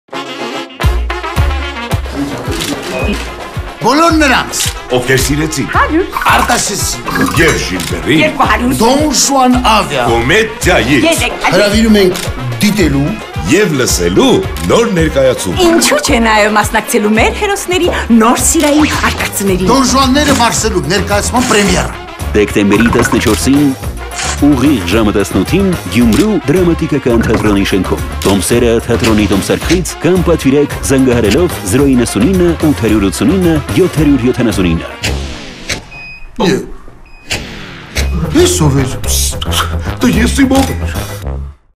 DON JUANNER@ MARSELUM... դեկտեմբերի 14-ին Գյումրու դրամատիկական թատրոնում
Հրանտ Թոխատյանը,
Նարեկ Դուրյանը,
Լալա Մնացականյանը
«ԴՈՆ ԺՈՒԱՆՆԵՐԸ ՄԱՐՍԵԼՈՒՄ» ներկյացումով
Գյումրու դրամատիկական թատրոնում՝ դեկտեմբերի 14-ին՝ ժամը 18:00:
Տոմսեր պատվիրեք՝ զանգահարելով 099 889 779...
PROF production